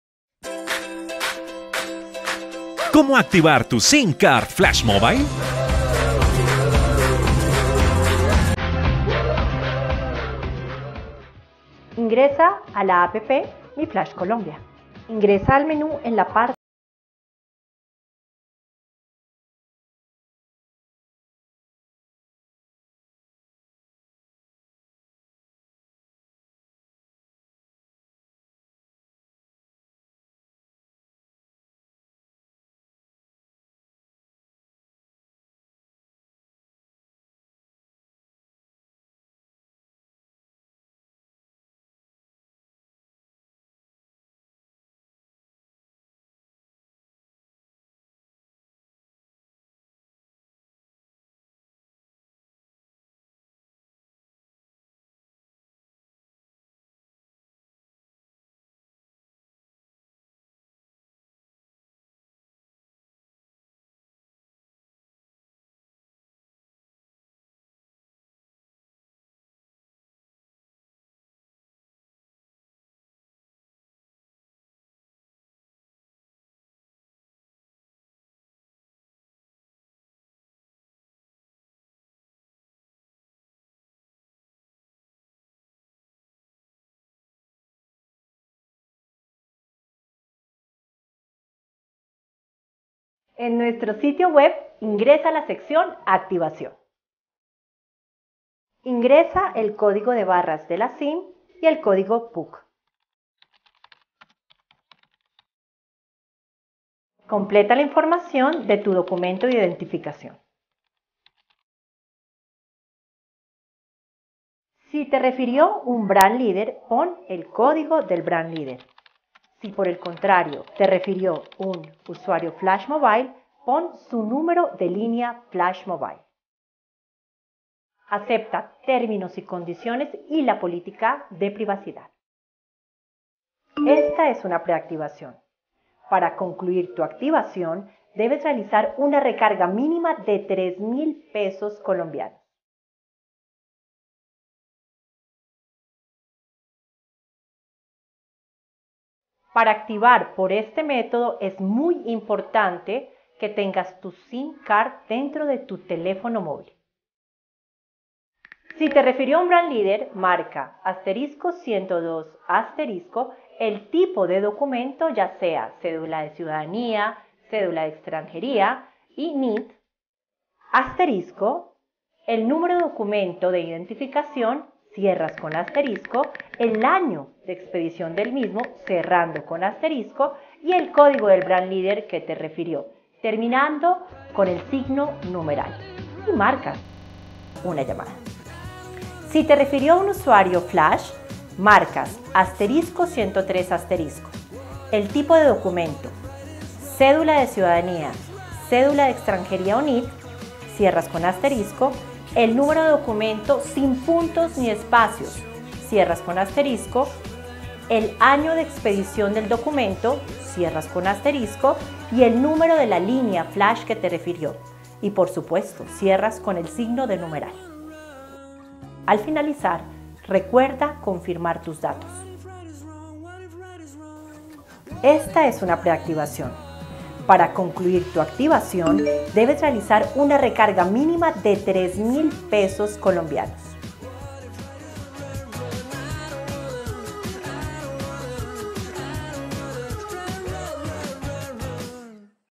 Como Activar Una Sim card Flash Mobile Colombia Aplicación y PC
FlashMobile En este vídeo quiero mostrar la manera mas fácil de activar la sim card desde la aplicación móvil y desde el pc, espero que sea de mucha ayuda y que compartan el vídeo con todos los de su equipo.

si por el contrario eres un cliente que quieres probar el servicio te dejo mi numero de whatsap para que que hagas contacto y hagamos la activación.